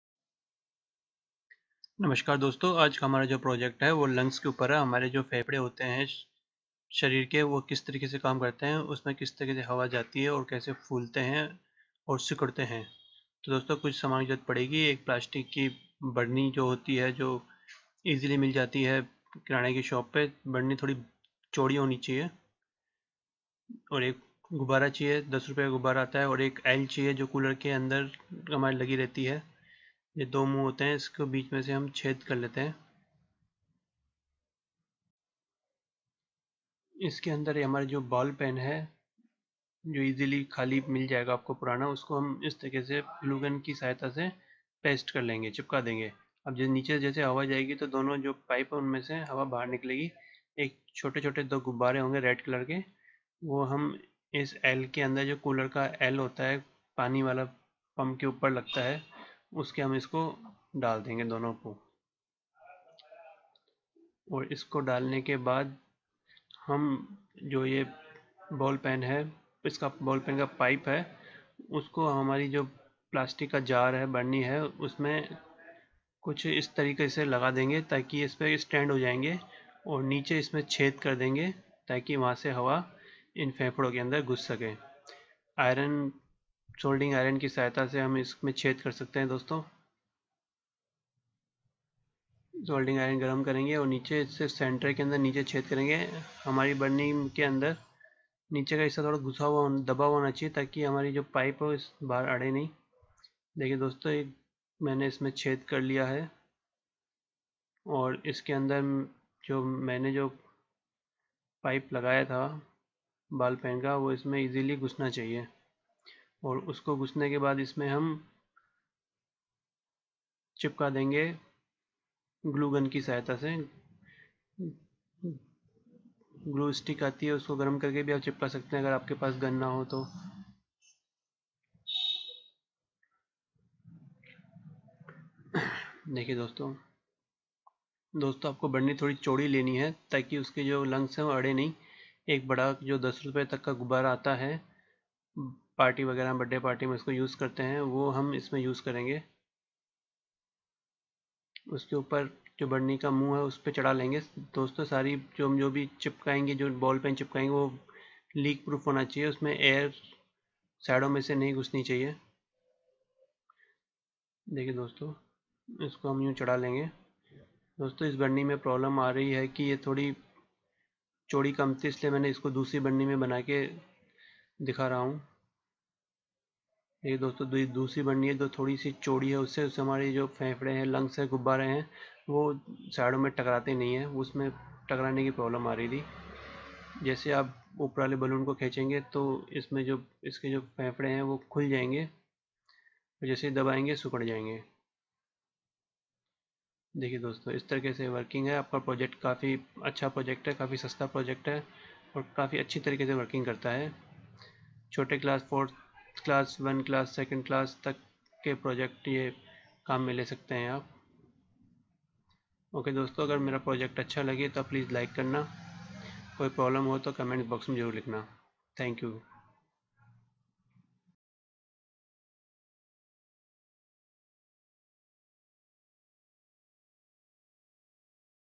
Lungs working model (hindi)
working model of the lungs with just a new plastic bottle and some balloons. You can see them inflating and deflating, breathing in and out. The Bottle is your chest , The Vertical of Y notch is the wind pipe, and two wings of Y are small wind pipes

All type of school project